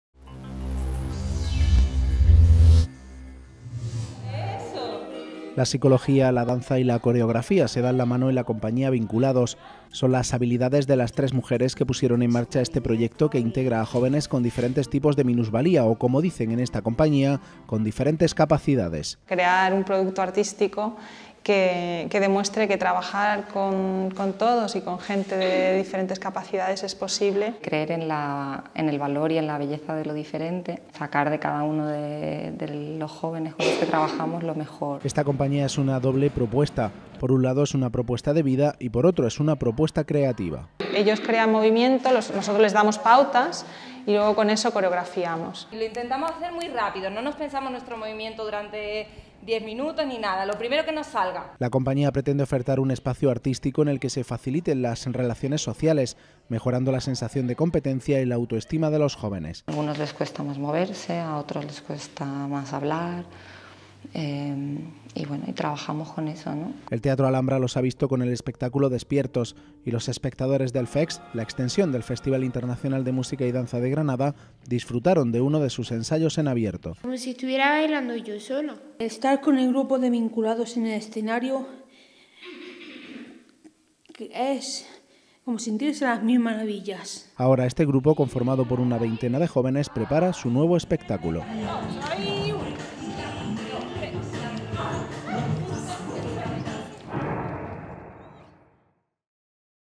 HDL Vinculados: danza y psicología como propuesta de vida para personas con diferentes capacidades
Una veintena de jóvenes con diferentes capacidades psíquicas y físicas conforman la compañía de danza Vinculados, en la que la psicología, la coreografía y la danza se dan la mano. Una propuesta creativa que al mismo tiempo es una propuesta de vida para los chicos y chicas que forman parte de esta iniciativa desarrollada en Granada.